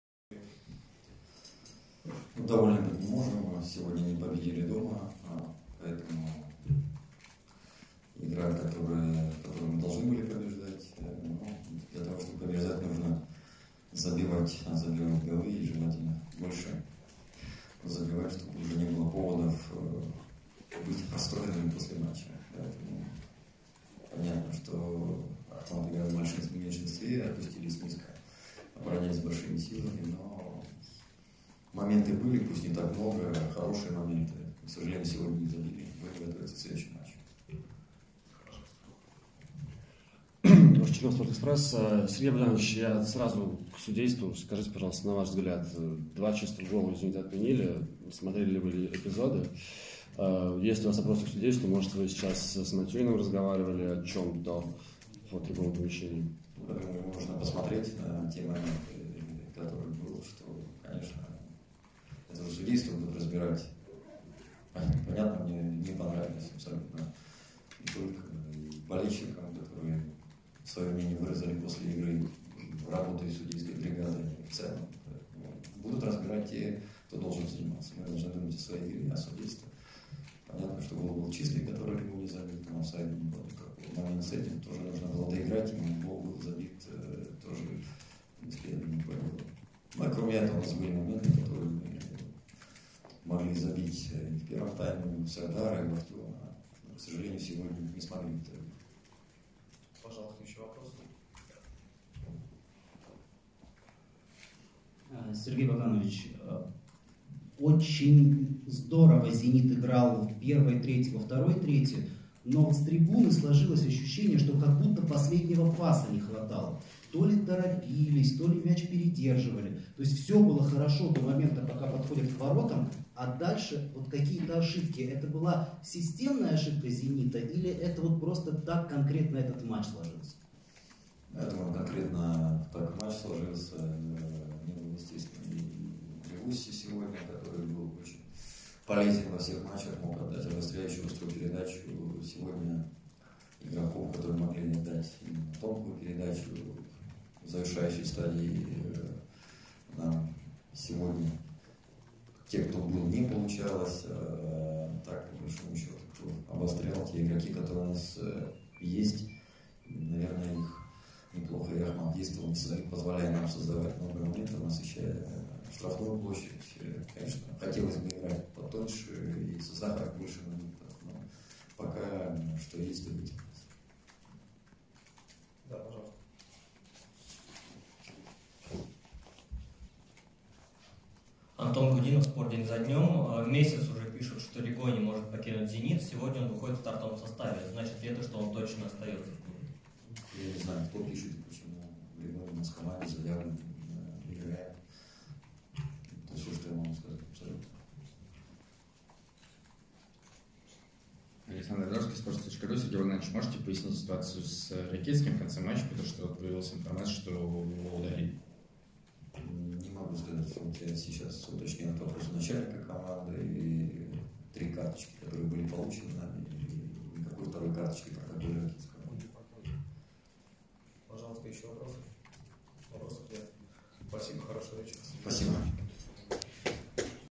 Сергей Семак: «Судейство Матюнина не понравилось абсолютно. Ригони забил чистый гол»
Главный тренер Зенита Сергей Семак подвел итоги матча с Ахматом